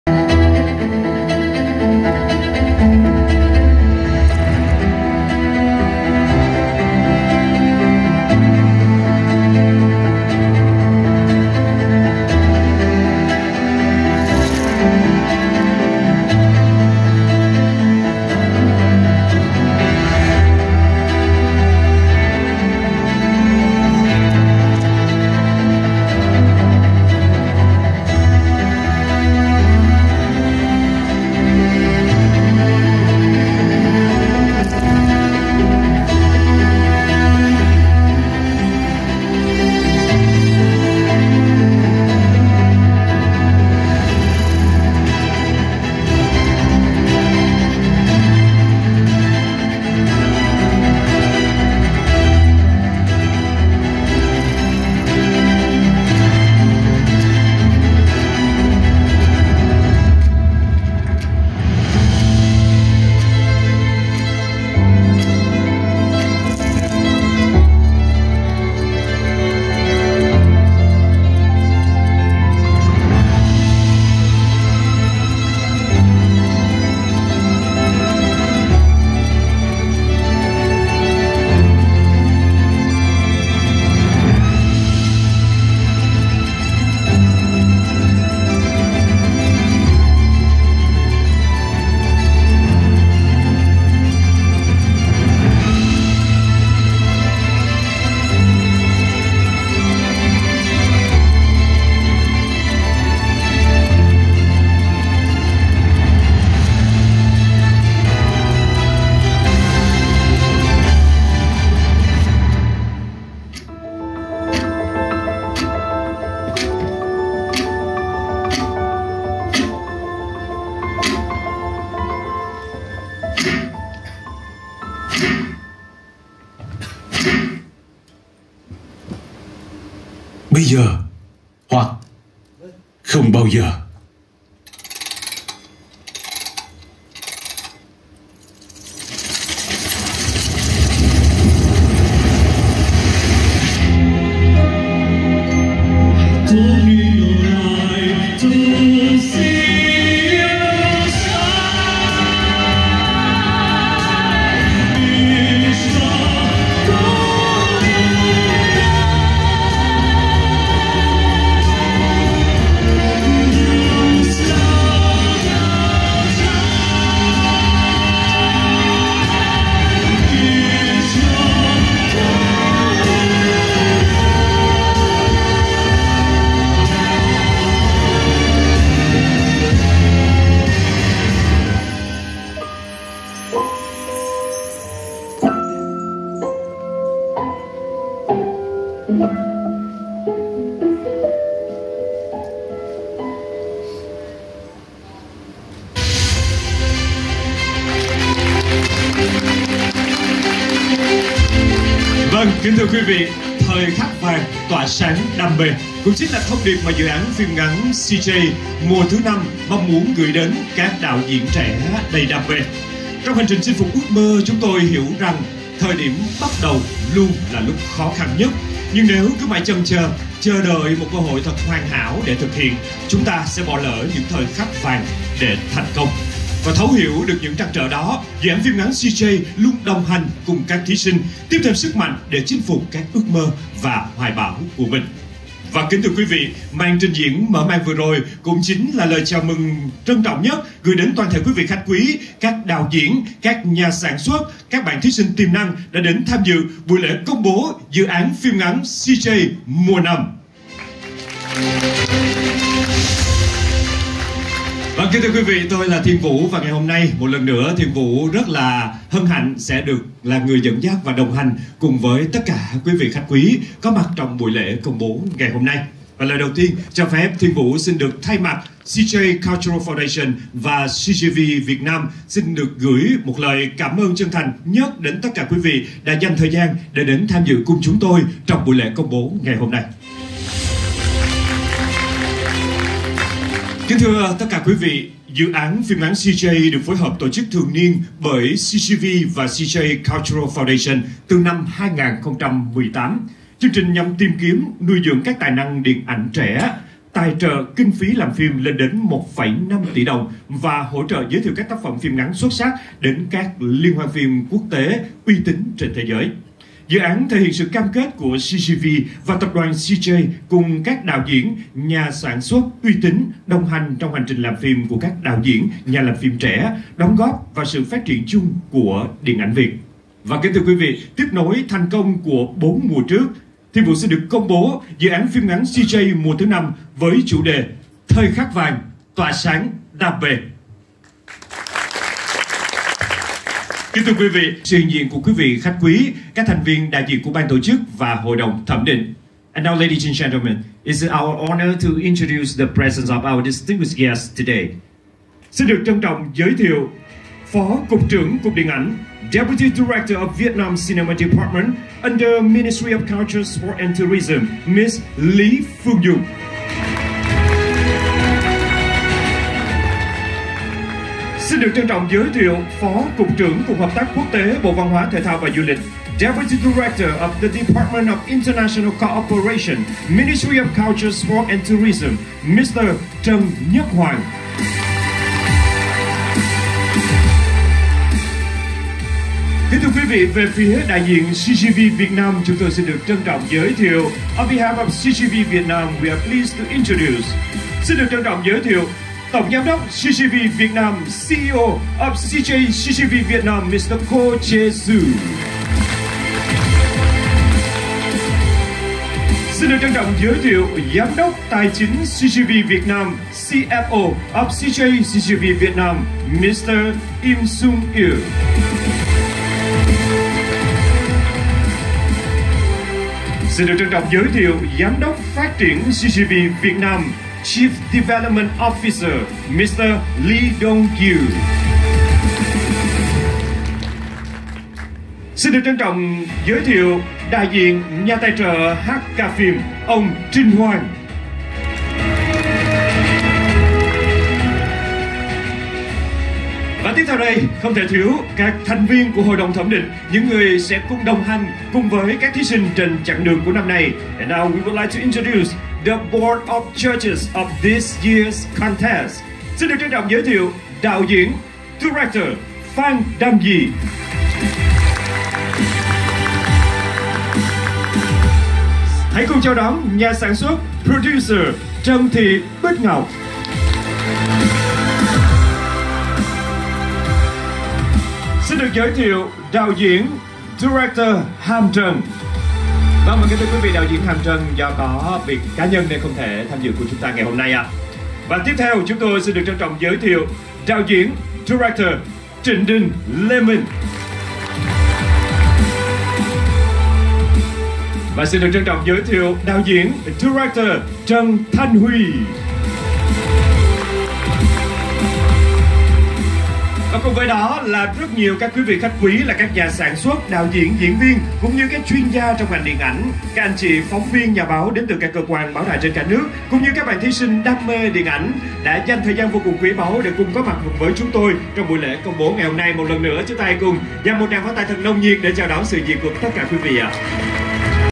Lễ Công bố Dự án phim ngắn CJ 2024 thu hút hàng trăm nhà làm phim trẻ
Lễ Công bố DựánphimngắnCJ 2024 thu hút hàng trăm nhà làm phim trẻ